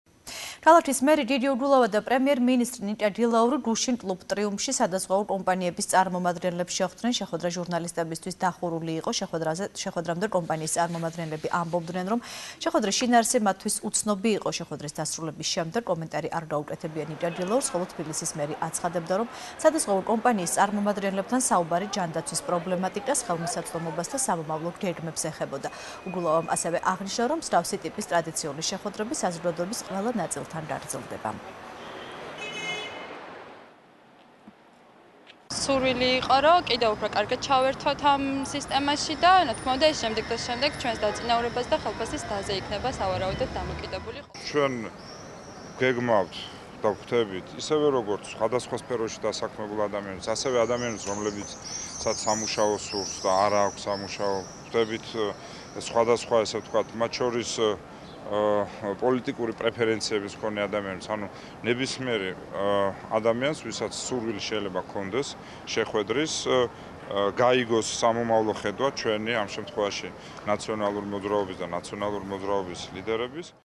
ახალი 12 | შეხვედრა "ტრიუმფში" | 16.06.12
ქალაქის მერი გიგი უგულავა და პრემიერ მინისტრი ნიკა გილაური, გუშინ, კლუბ "ტრიუმფში" სადაზღვევო კომპანიების წამორმადგენლებს შეხვდნენ. შეხვედრა ჟურნასლიტებისთვის დახურული იყო. შეხვედრამდე კომპანიის წარმომადგნლები ამბობდნენ, რომ შეხვედრის შინაარსი მათთვის უცნობი იყო. შეხვედრის დასრულების შემდეგ კომენტარი არ გაუკეთებია ნიკა გილაურს, ხოლო თბილისის მერი აცხადებდა, რომ სადაზღვევო კომპანიის წარმოამდგნლებთან საუბარი ჯანდაცვის პრობლემატიკას, ხელმისაწვდომობას და სამომავლო გეგმებს შეეხებოდა. უგულავამ ასევე აღნიშნა, რომ მსგავსი ტიპის ტრადიციული შეხვედრები საზოგადოების ყველა ნაწილთან გაგრძელდება.